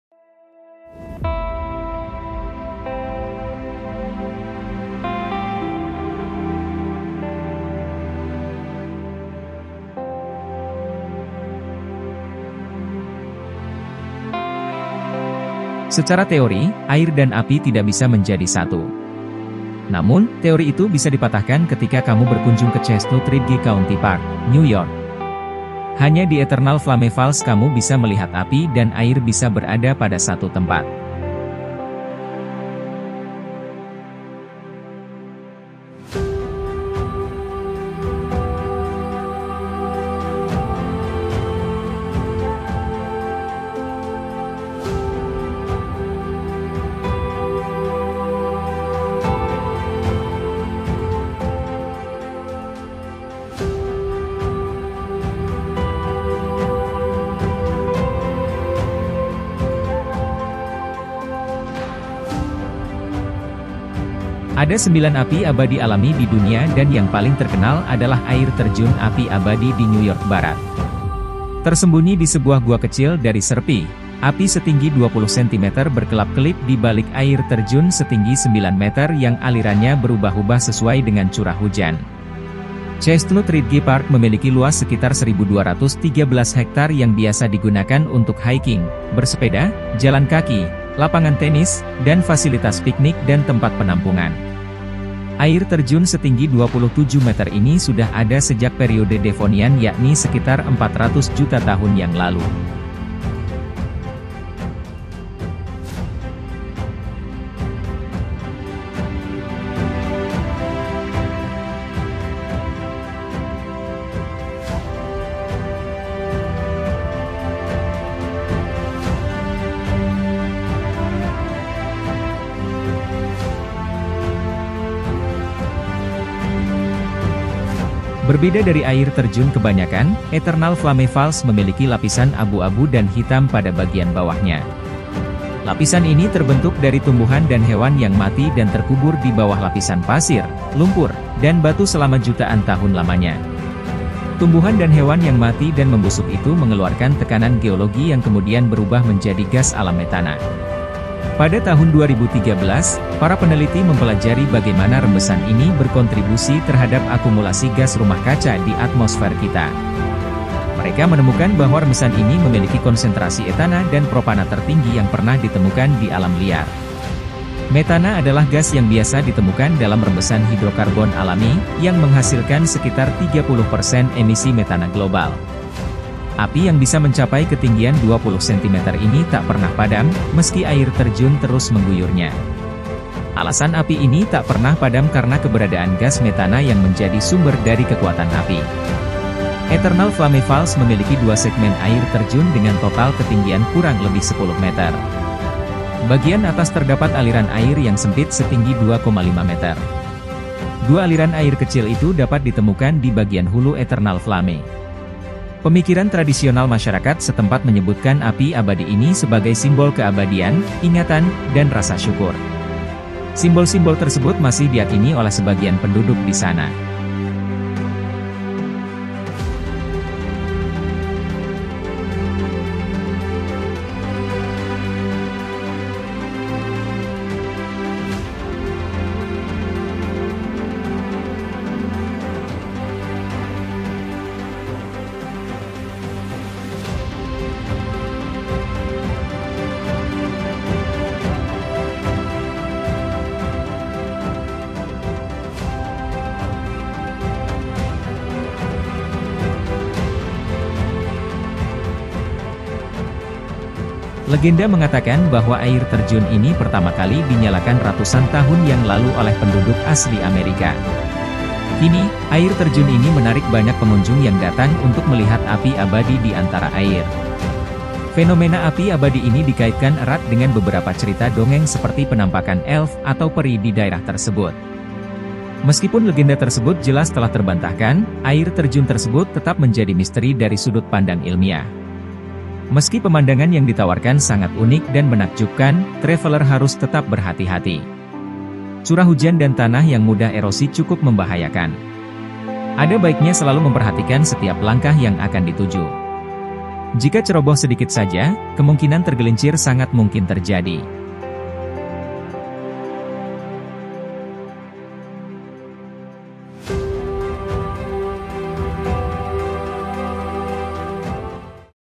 Api Abadi di Air Terjun
Ada 9 api abadi di dunia dan yang paling terkenal adalah air terjun api abadi di New York Barat. Tersembunyi di sebuah gua kecil dari serpih. Api setinggi 20 cm berkerlap-kerlip dai balik air terjun.